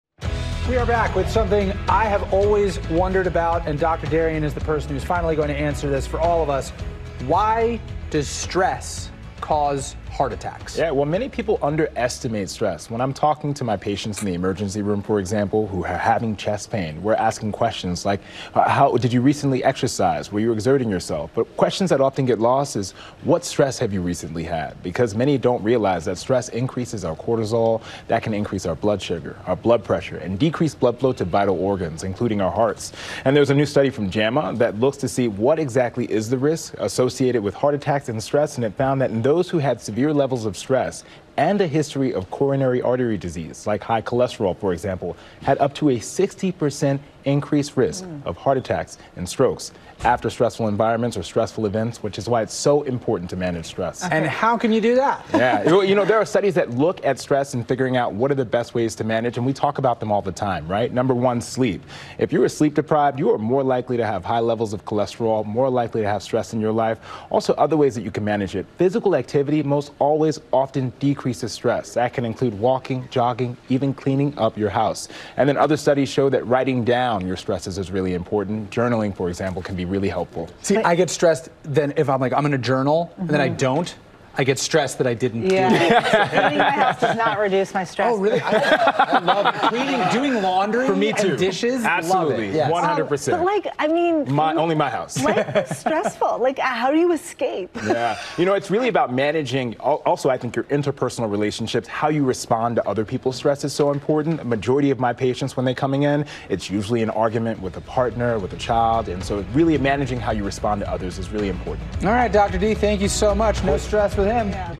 Stress response increases heart risks by up to 60%, study shows
A new study finds that stress responses increase heart attack, heart failure or death risks by up to 60% in people with heart disease. Dr. Darien Sutton explains.